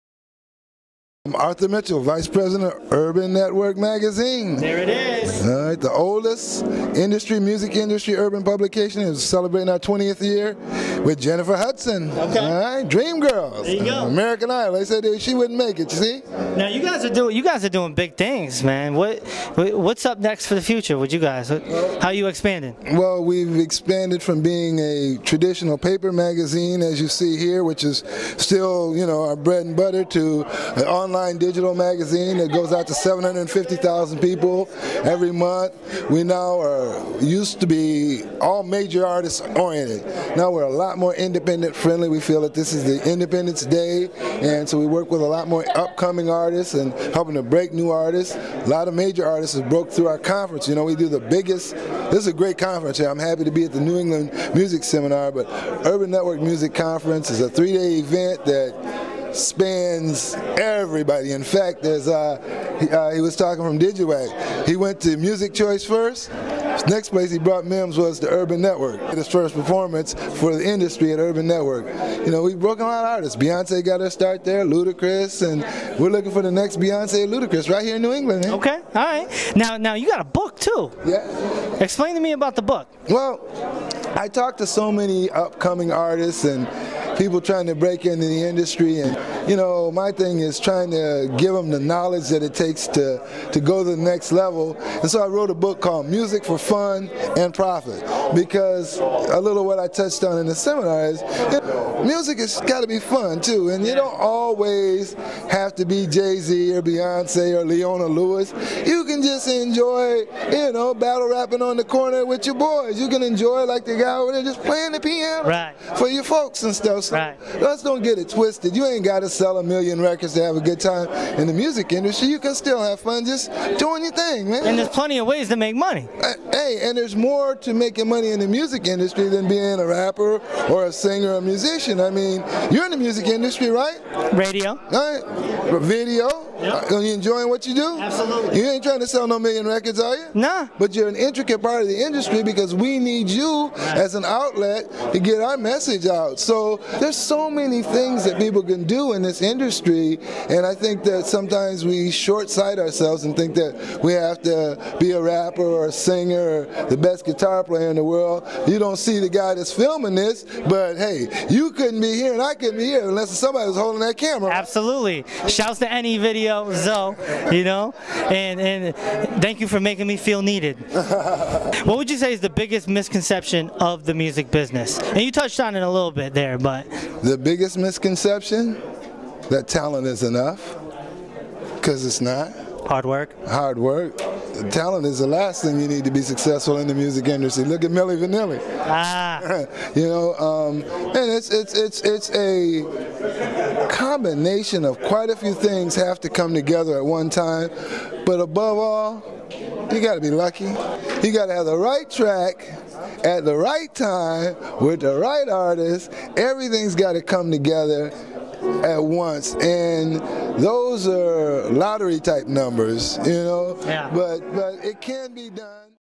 Arthur Mitchell of Urban Network Magazine Interviewed by K-dubasis Runnindis
Arthur Mitchell VP of Urban Network Magazine stops by to talk with K-dubasis Runnindis at the 2008 New England Music Seminar